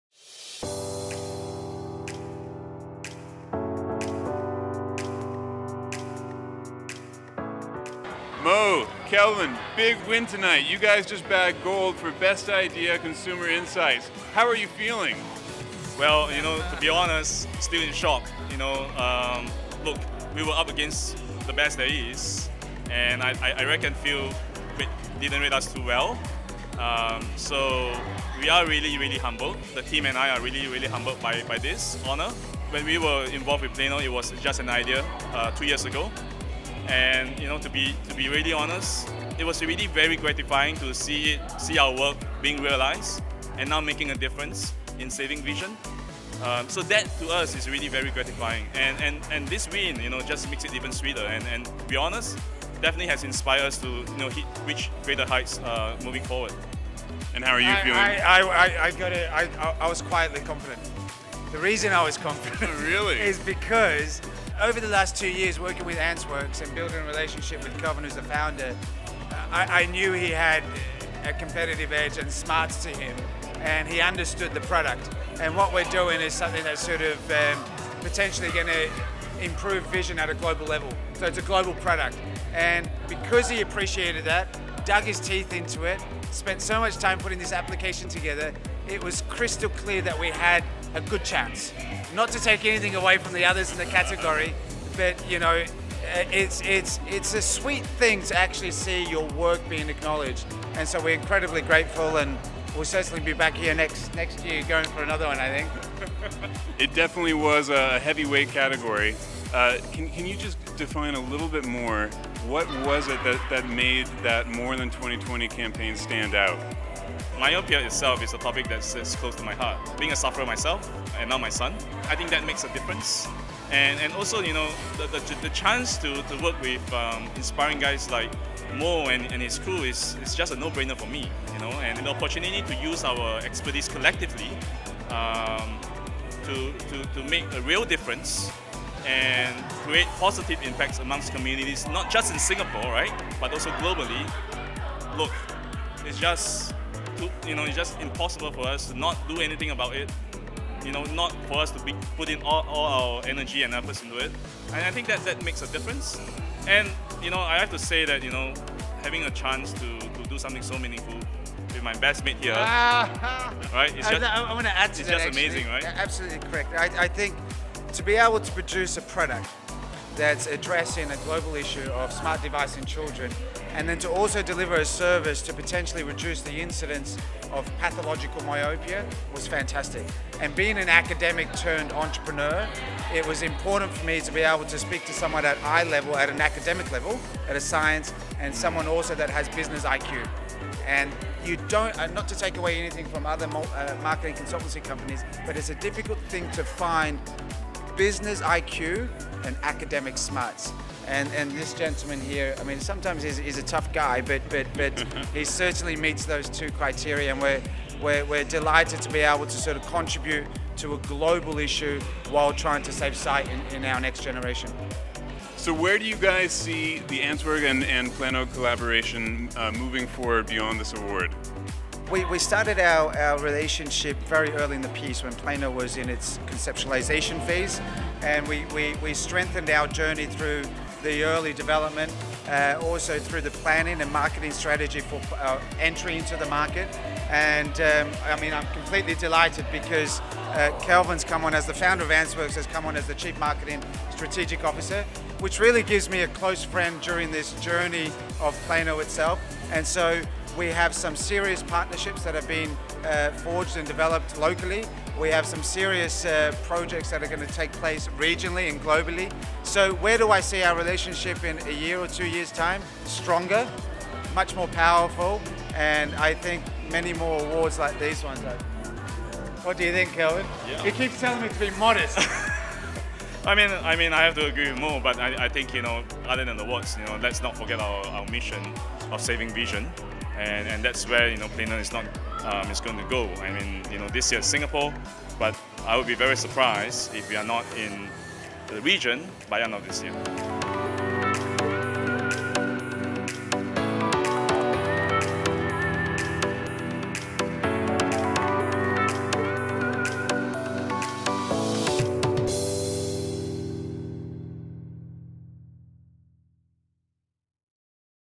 MARKies Awards Singapore 2018 - Interview with Antzworkz Consultants and plano
Catch the interview from the Gold winner of the category Best Idea – Consumer Insight!

The MARKies awards celebrate the most innovative, creative and effective campaigns or projects spanning Singapore’s entire marketing services industry. Do not miss the highlights of its second year as a standalone awards show that took place on 27 April 2018 at Sofitel Singapore City Centre.